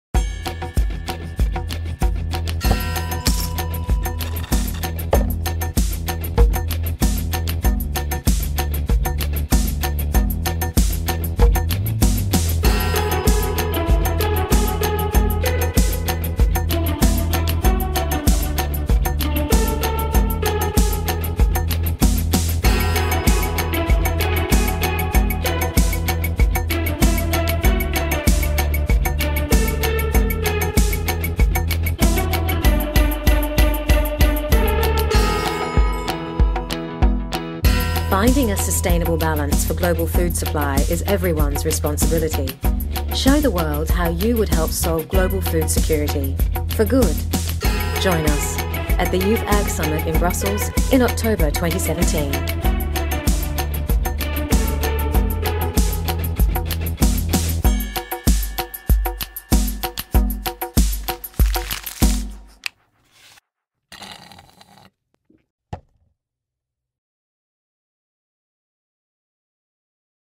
Youth Ag Summit
Mostre ao mundo como você resolveria a segurança alimentar global para sempre. Venha participar da Youth Ag-Summit 2017!